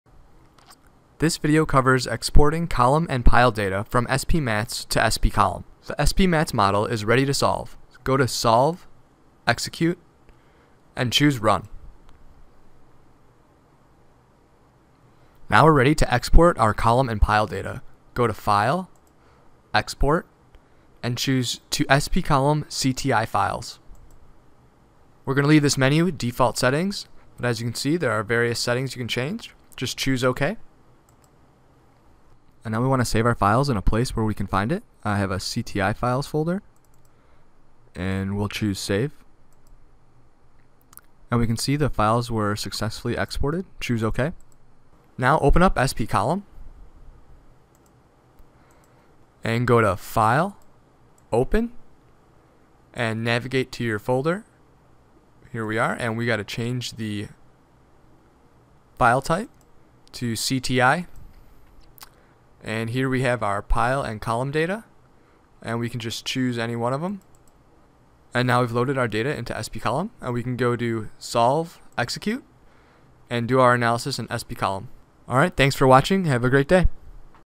StructurePoint - Exporting Column and Pile Data from spMats v8.50 to spColumn
This tutorial video shows how to export column and pile data from spMats v8.50 model to spColumn.